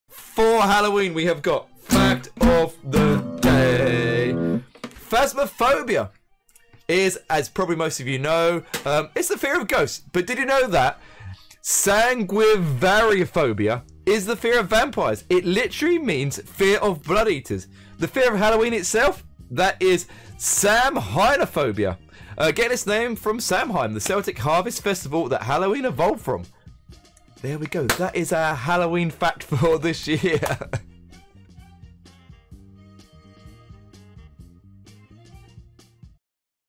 Halloween Fears: From Phasmophobia to Samhainophobia. FACT!
Explore the world of supernatural phobias! From fear of ghosts to dread of vampires and even Halloween itself, discover the intriguing origins behind these unique fears.

I am live every Monday, Wednesday and Friday night from 8pm UK time.  Come along for Facts, Beer of the day, Death shots, and gameplay. used by prod. Difference studio and beats